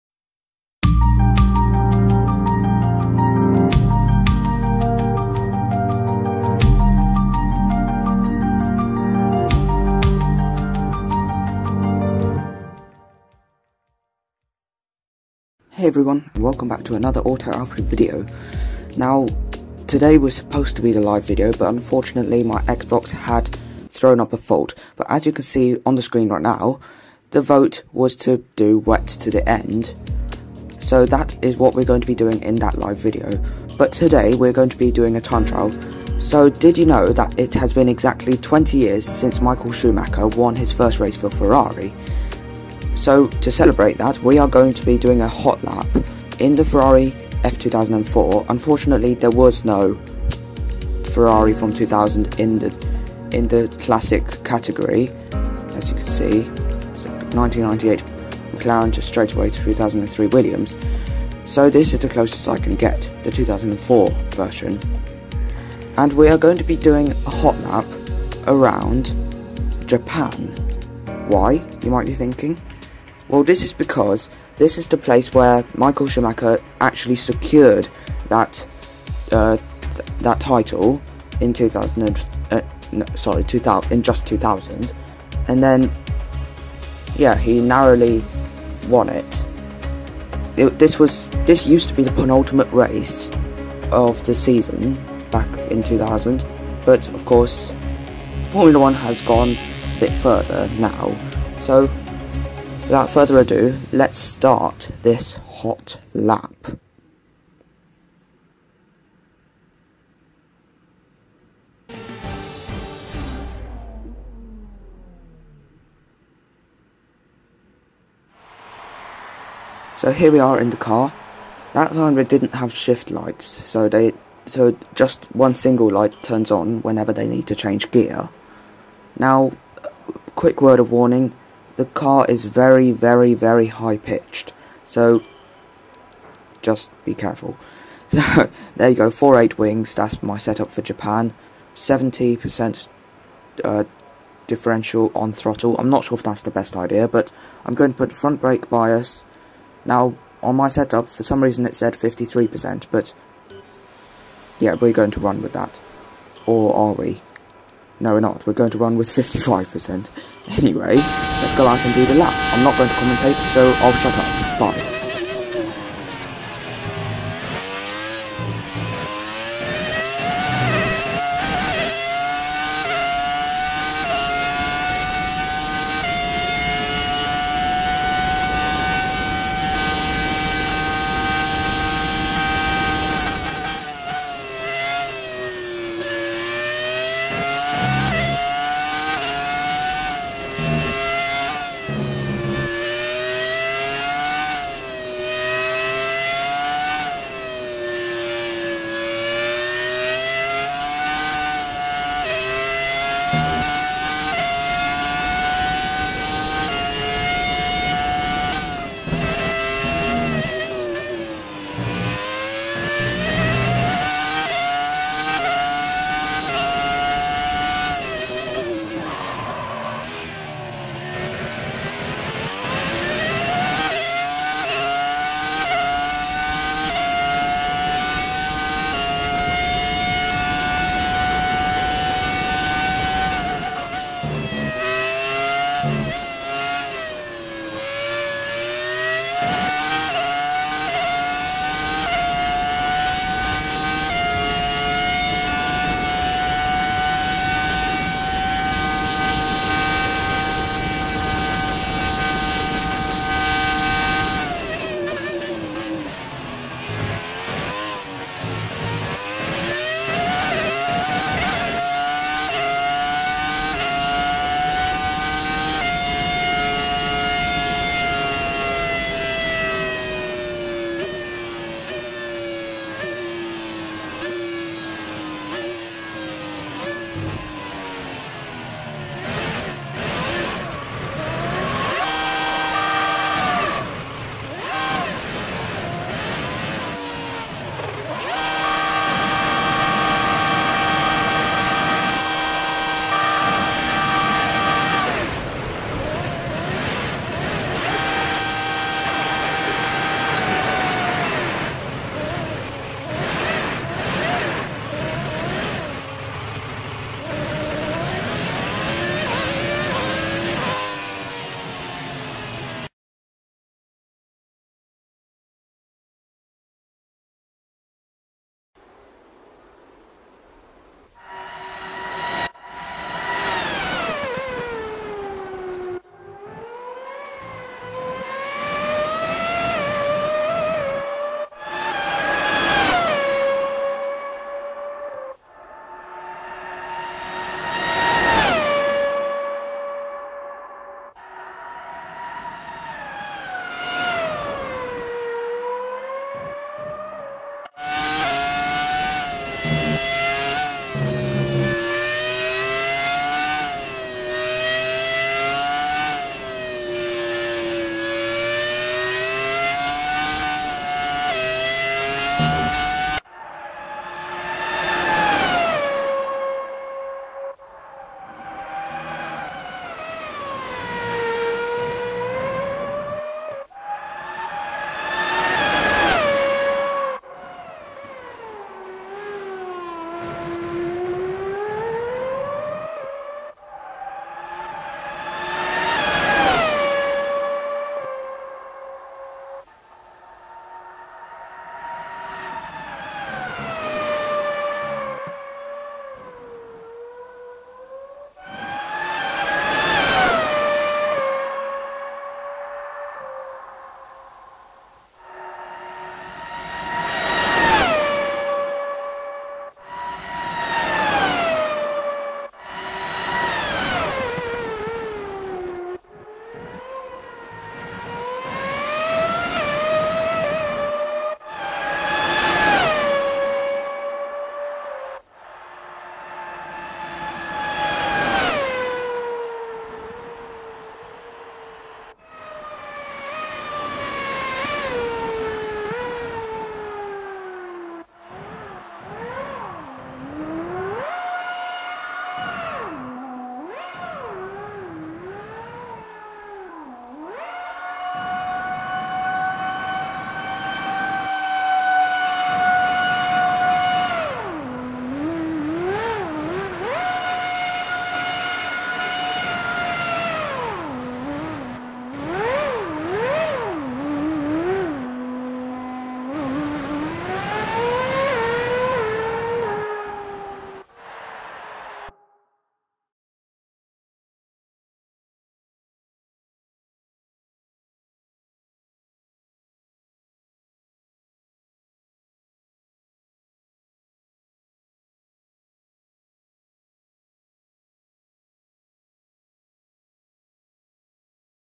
20th Anniversary Hotlap for Michael Schumacher
It has been 20 years since Michael Schumacher won his first of many Drivers' Championships for Ferrari. To celebrate that, I am going to be putting that Ferrari back onto where it belongs... the track! I will be driving the Ferrari F2004 (not the exact car that he won that first championship with Ferrari in), the car which gave him his last Drivers' Championship win. It will be taken to Suzuka, the place where he secured that title, for a hotlap!

Equipment used:
Logitech G920 (for Xbox and PC)